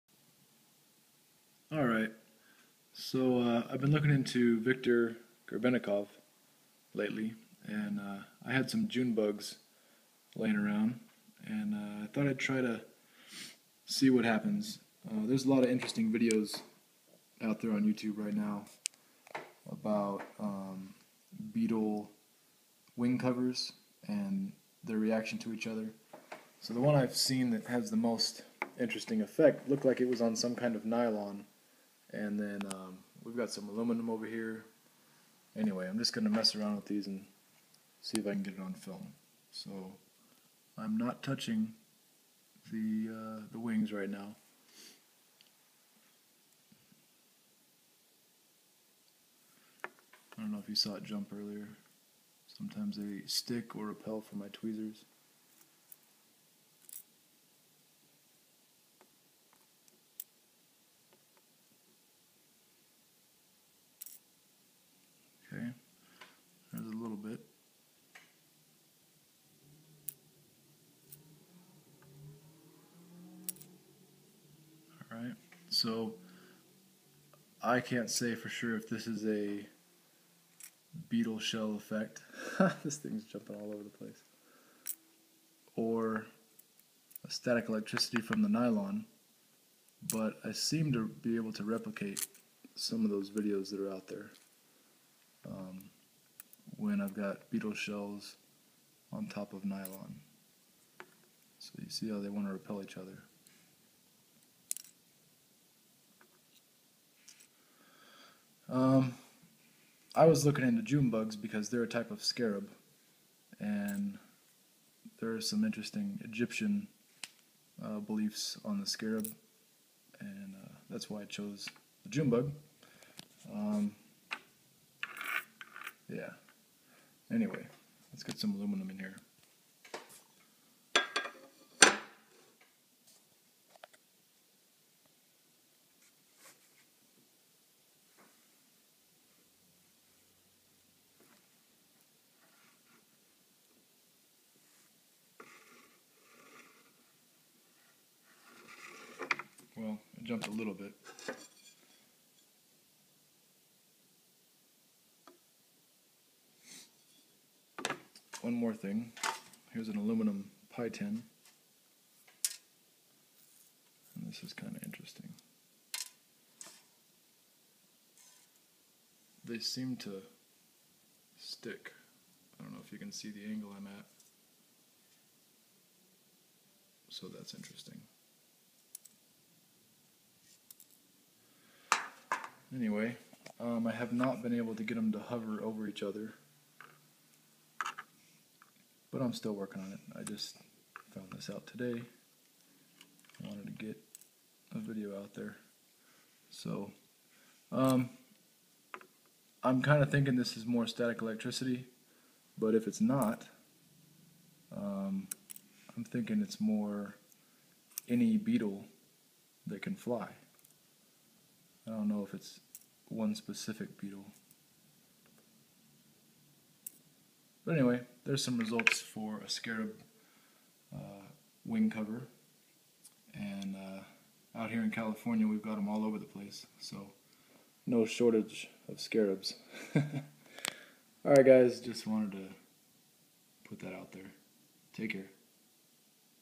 Viktor Grebennikov #1 - Testing Scarab (June Bug) Wing Covers
If you enjoy my videos, Please consider contributing to my research.

Inspired by Viktor Grebennikov and TheRealVerbs.

This video demonstrates effects of Scarab (June Bug) wing covers reacting to metal and each other while on top of nylon. After seeing many videos on this phenomenon, I decided to attempt to replicate the results for myself. So far I have been able to replicate them repelling each other up to 4-5", stick to & repel from stainless steel, & stick to aluminum. I would like to see them hover one above the other, but have not been able to accomplish this yet. To me this looks like a STATIC ELECTRICITY effect from the nylon because it only works while they are on the nylon. I will try these and other wing covers on other materials as the research continues.

CONCLUSION:
Videos that show this effect while using any of the following are hoaxes and misleading (nylon, plastic, plexiglas, ect...) These shells will not show this effect on grounded materials. My research will continue with this in mind.

As crazy as this sounds, the point of this research is to be able to replicate the flying platform made by Viktor Grebennikov. This would involve identifying the nonostructures that cause the hover effect and possibly have them replicated onto sheets. Viktor claimed he used beetle shells to create a flying platform that could reach speeds up to 1500 mph without feeling the effects of inertia, wind, or air friction. For those viewers who do not know anything about this research, please visit this website for more information.

Please leave your comments & suggestions. Constructive criticism is welcomed.

Russell